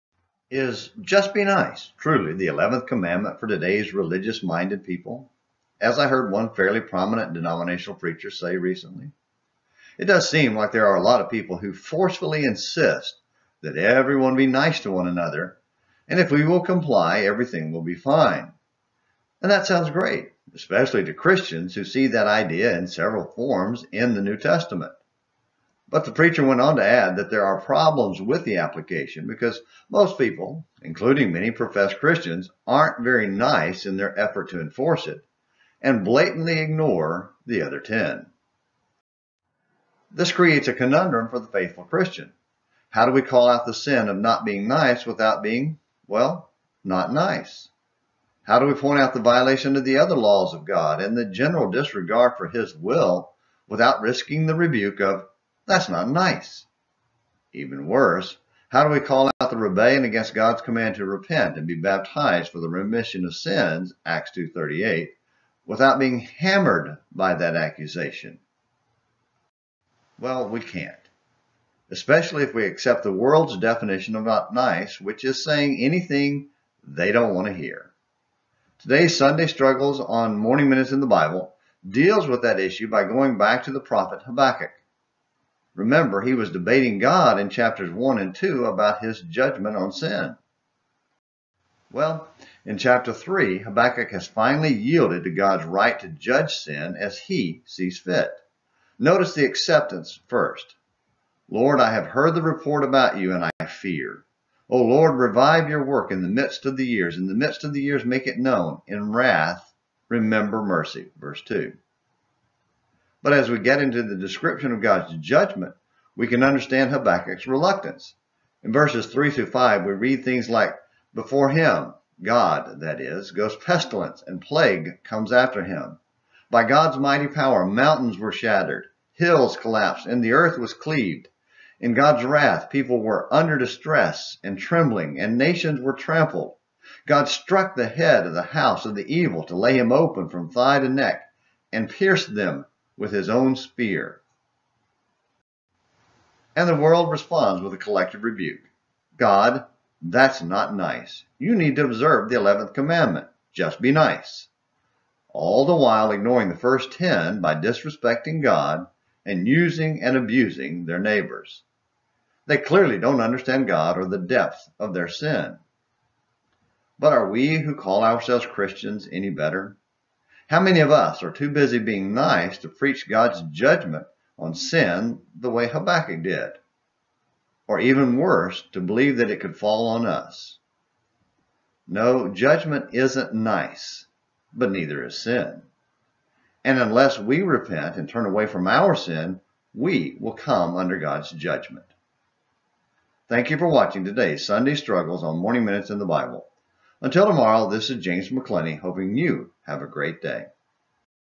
God’s Judgment Isn’t “Nice” Habakkuk 3
Is “Just be nice!” truly the 11th commandment for today’s religious minded people, as I heard one fairly prominent denominational preacher say recently? It does seem like there are a lot of people who forcefully insist that everyone be nice to one another. If we will comply everything will be fine. And that sounds great, especially to Christians who see that idea in several forms in the NT. But the preacher went on to add that there are problems with the application because most people, including many professed Christians, aren’t very nice in their effort to enforce it, and blatantly ignore the other 10.

This creates a conundrum for the faithful Christian. How do we call out the sin of not being nice without being, well, not nice? How do we point out the violation of the other laws of God and the general disregard for His will without risking the rebuke of “That’s not nice”? Even worse, how do we call out the rebellion against Gods’ command to “repent and be baptized for the remission of sins” (Acts 2:38) without being hammered by that accusation? Well we can’t! Especially if we accept the world’s definition of “not nice”, which is saying anything they don’t want to hear.

Today’s Sunday's Struggles on Morning Minutes in the Bible deals with that issue by going back to the prophet Habakkuk. Remember he was debating God in chapters 1 & 2? Well in chapter 3 Habakkuk has finally yielded to God’s right to judge sin as He sees fit. Notice the acceptance first. “Lord, I have heard the report about you and I fear. O Lord, revive your work in the midst of the years, in the midst of the years make it known; in wrath remember mercy. ” (v2). But as we get into the description of God’s judgment we can understand Habakkuk’s reluctance. In verses 3-15 we read things like “Before Him (God that is) goes pestilence, and plague comes after Him”. By God’s mighty power “mountains were shattered”, “hills collapsed”, and “the earth was cleaved”. In God’s wrath people were “under distress” and trembling” and “nations were trampled”. God “struck the head of the house of the evil to lay him open from thigh to neck” and “pierced” them with His own spear”.

And the world responds with a collective rebuke, “God, that’s not nice! You need to observe the 11th commandment – Just be nice!” All the while ignoring the first 10 by disrespecting God and using and abusing their neighbors. They clearly don’t understand God or the depth of their sin. But are we who call ourselves Christians any better?  How many of us are too busy being nice to preach God’s judgment on sin the way Habakkuk did? Or even worse to believe that it could fall on us? No judgment isn’t nice, but neither is sin. And unless we repent and turn away from our sin we will come under God’s judgment.